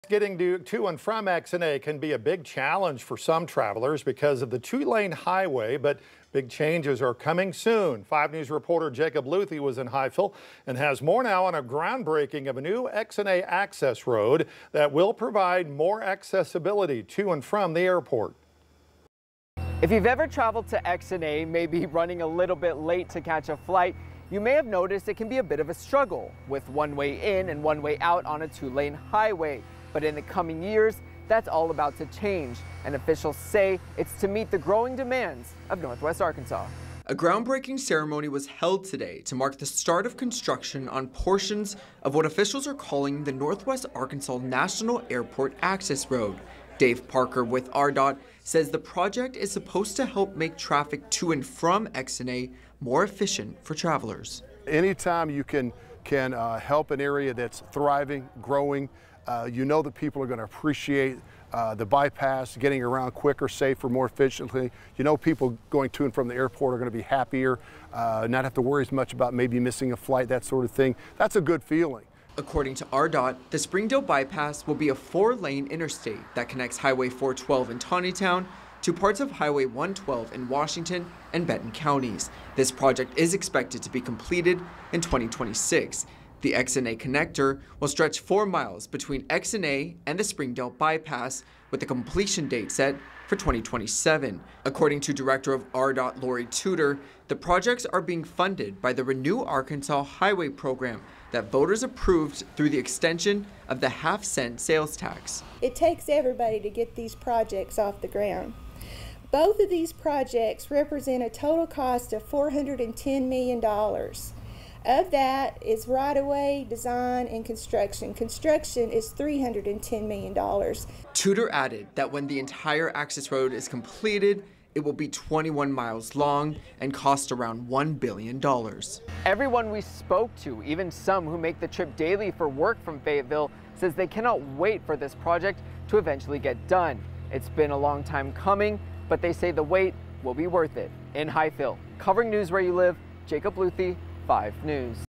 Construction begins on XNA access road
Dave Parker with ARDOT says the project is supposed to help make traffic to and from XNA more efficient for travelers.